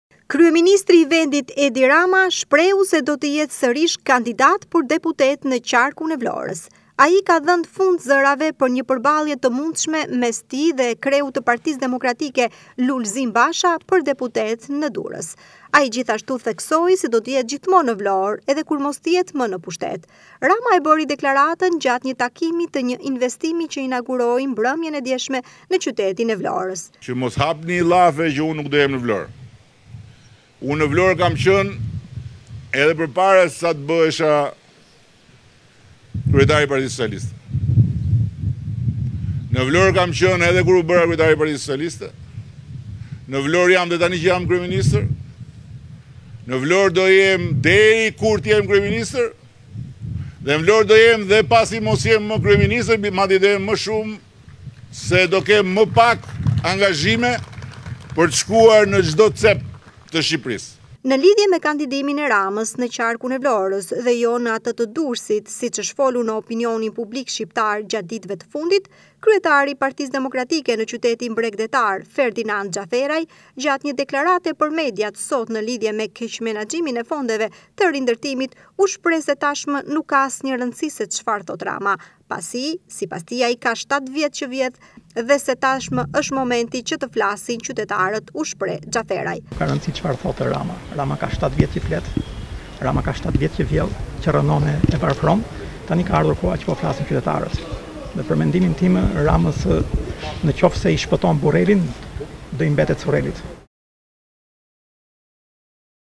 Durrës Rama konfirmon kandidimin për deputet në qytetin e Vlorës, PD Ka shtatë vjet që vjedh
Durrës Rama konfirmon kandidimin për deputet në qytetin e Vlorës, PD Ka shtatë vjet që vjedh, tani është momenti  të flasin qytetarët